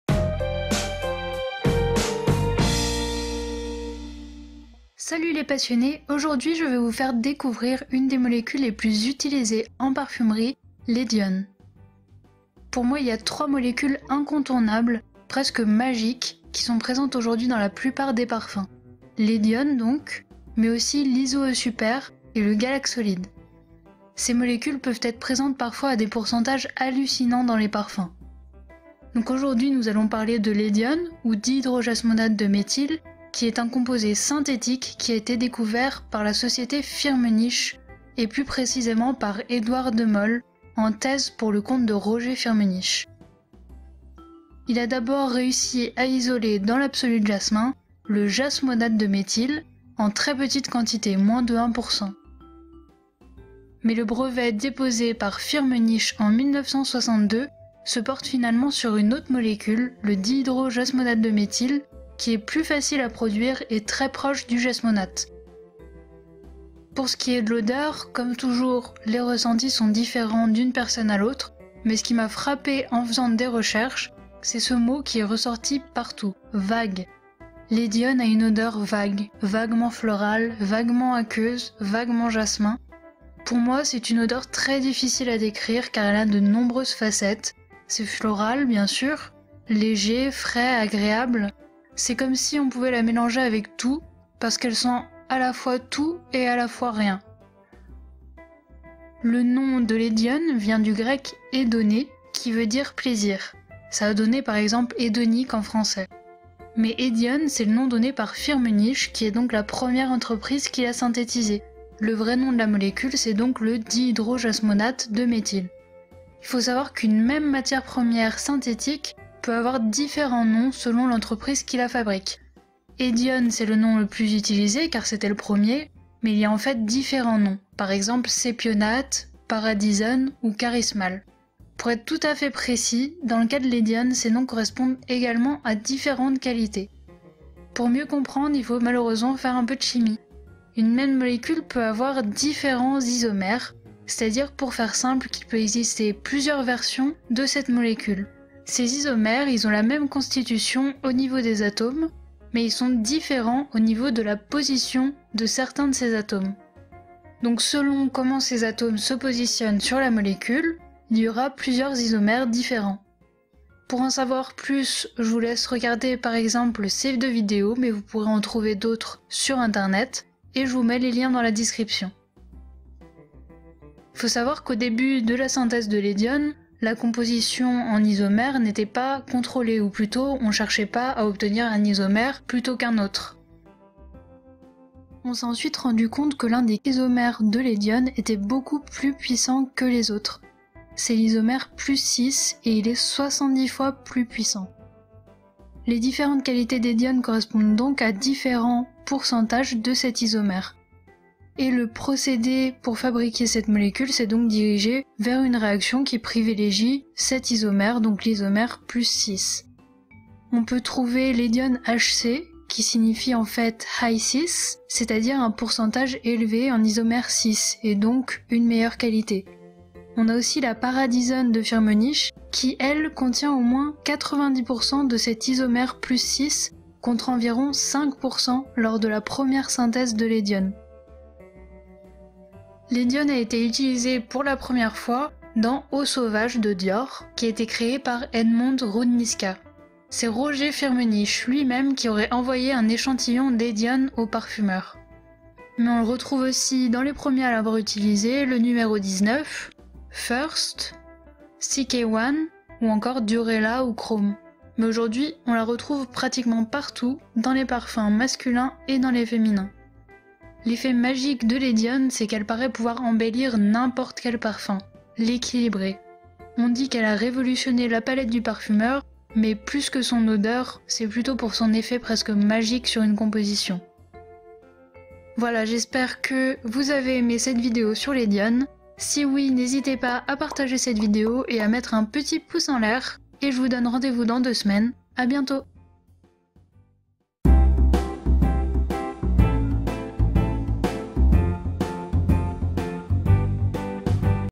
L'hédione, molécule magique
Découvrez l'Hedione, une des molécules les plus utilisées en parfumerie !

RDV mardi 18h30 dans deux semaines pour une nouvelle vidéo :)

Si vous avez aimé n'hésitez pas à mettre un pouce en l'air !

Toute critique constructive est appréciée dans les commentaires ;)

"Nez" la revue olfactive, 1ère édition Printemps/été 2016